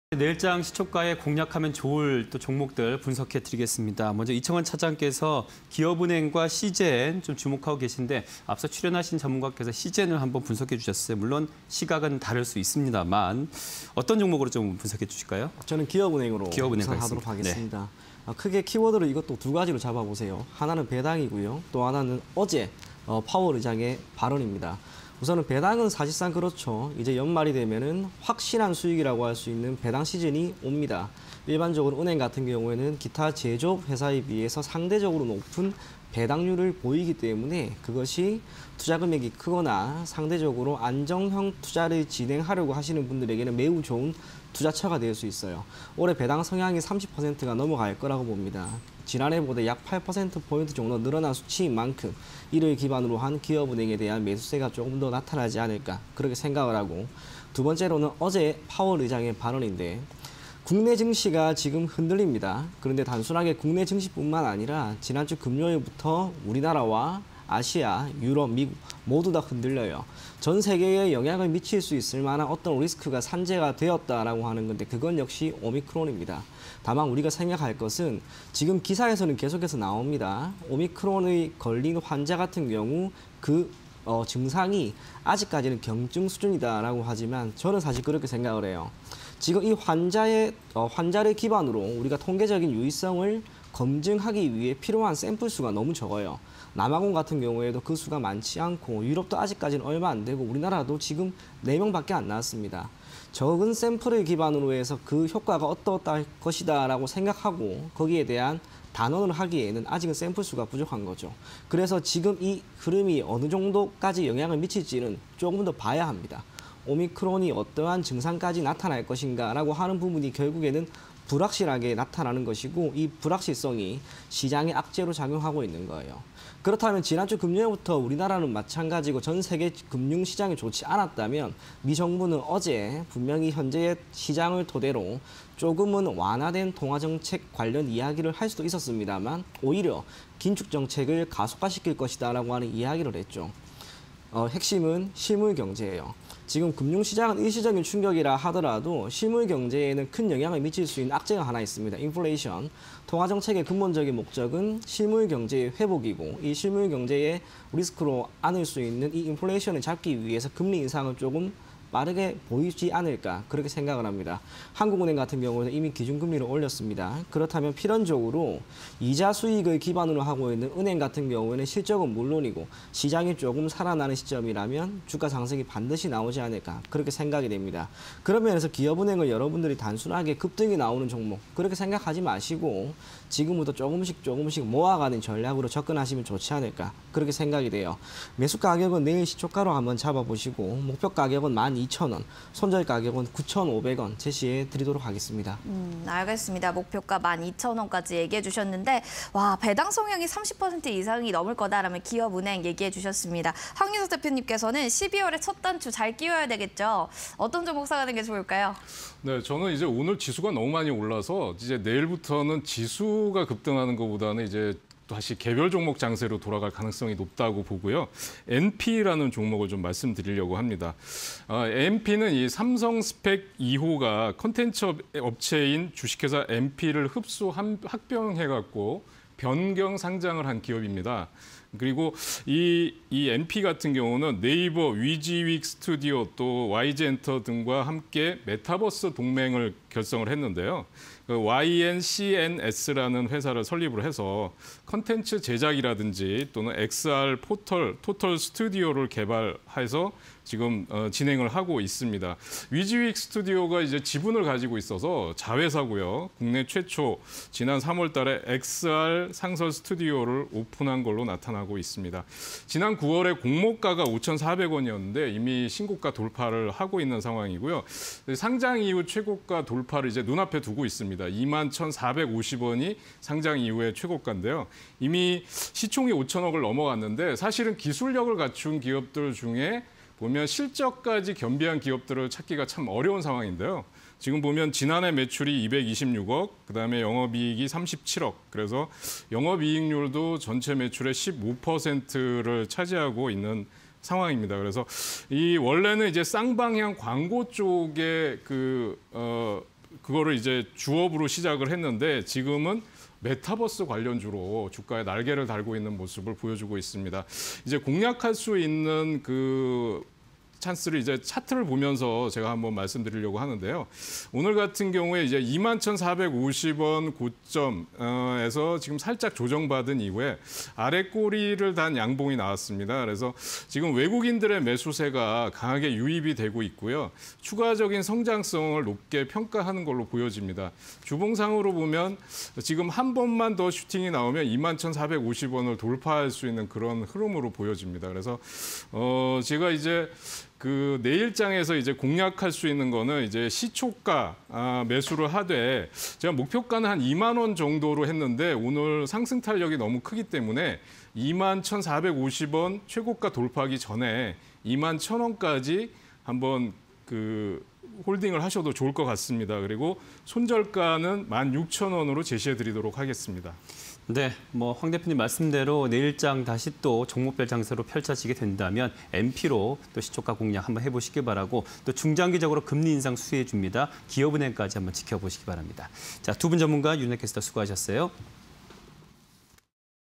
기업은행(024110), 엔피(291230)_사가지 (20211201)
기업은행(024110)
- 미국의 긴축 정책 시사
- 추가적 금리 인상 기대감
- 30% 수준 웃도는 배당 성향

엔피(291230)
- 삼성스팩2호, '엔피' 흡수합병… 변경 상장
- NAVERㆍ위지윅스튜디오 등과 메타버스 '동맹'
- 지난 3월, 국내 최초 XR상설스튜디오 오픈